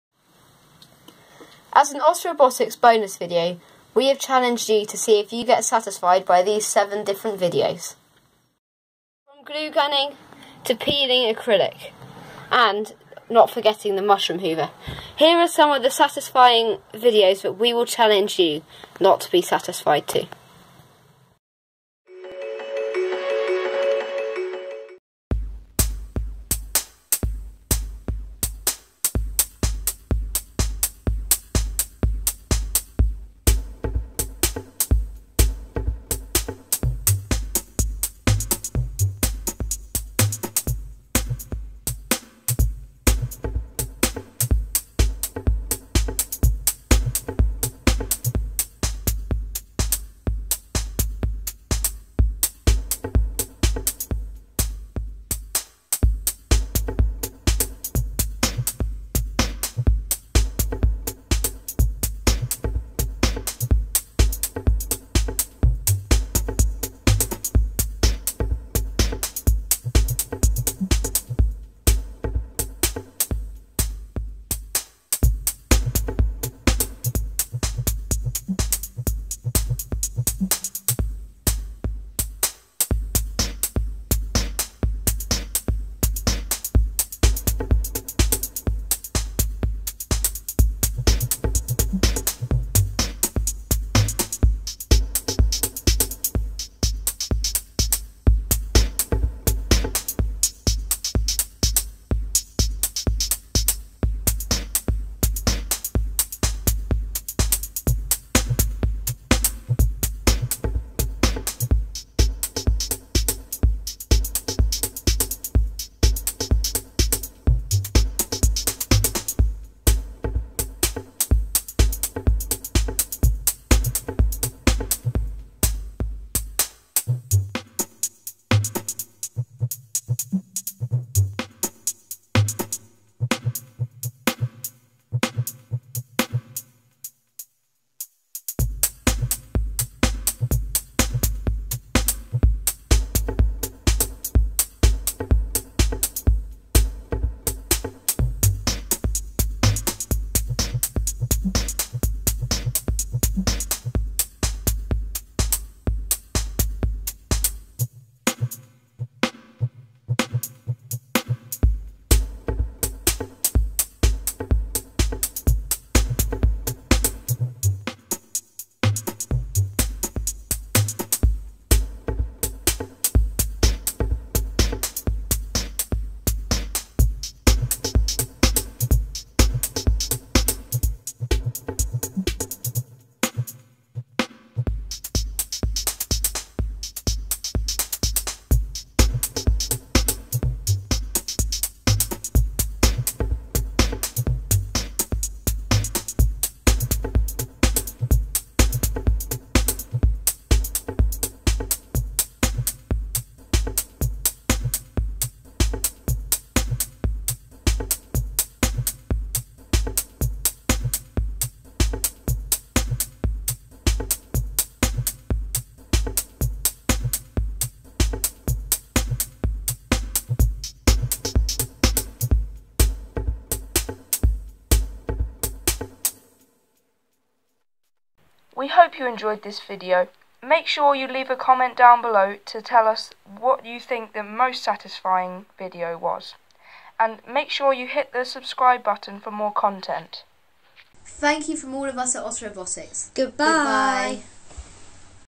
Bonus Video -- Try not to be satisfied (Otter Robotics Edition)
Here is another bonus video. This one has to be a record as we came up with an idea, filmed the video and had it edited in 3 hours.